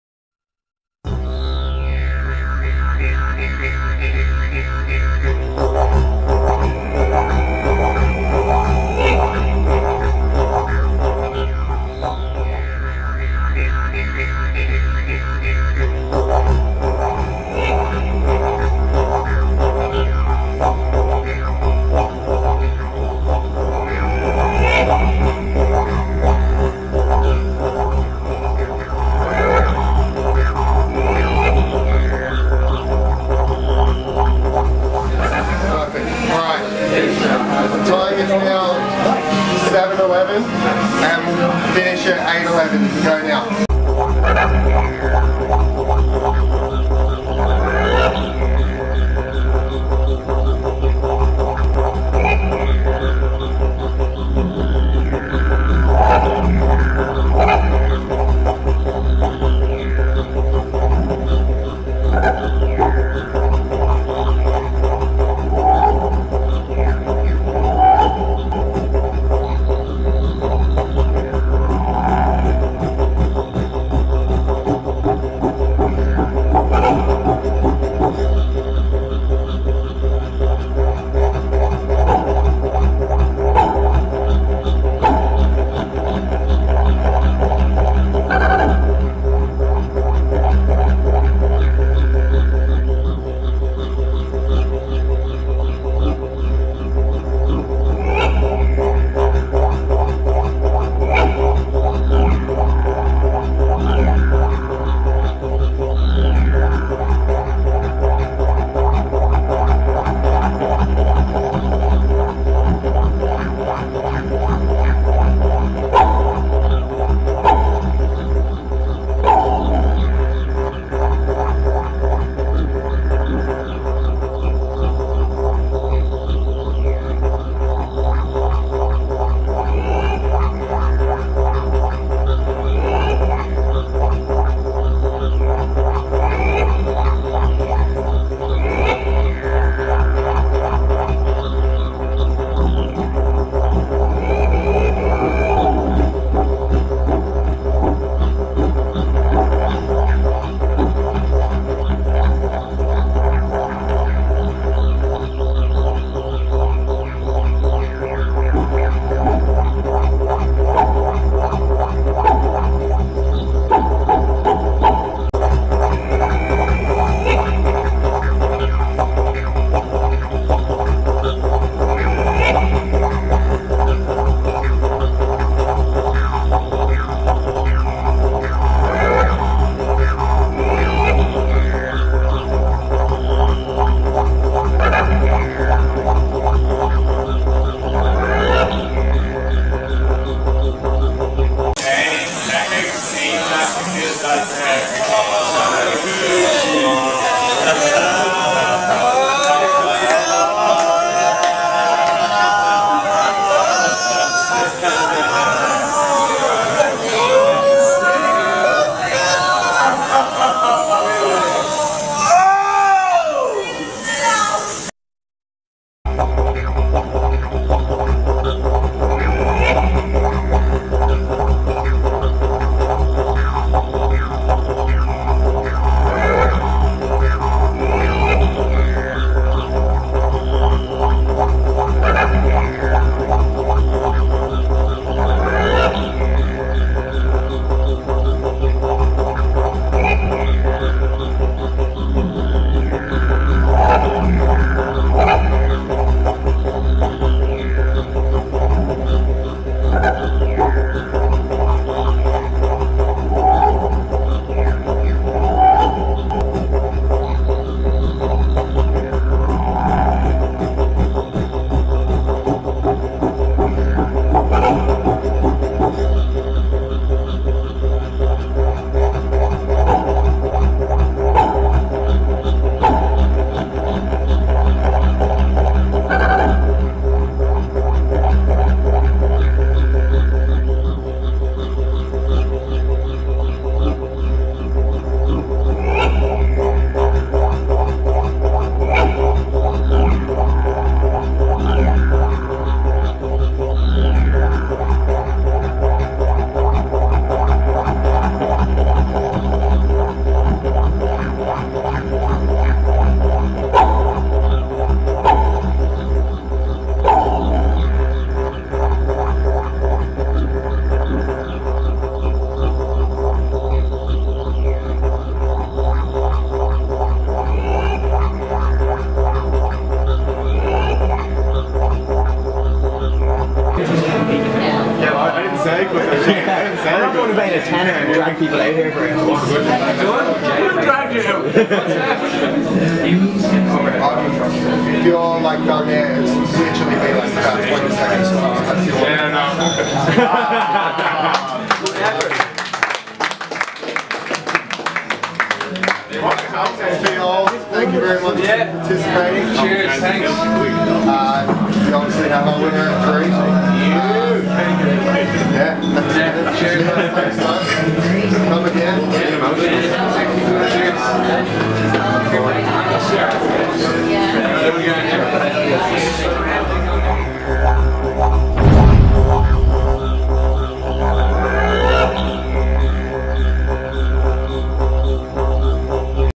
Lust for Gluttony: Aussie BBQ Great White Challenge
This is a challenge that The Aussie BBQ hold once a month. It is €10 in and that is it,
all you have to do then is eat all you can.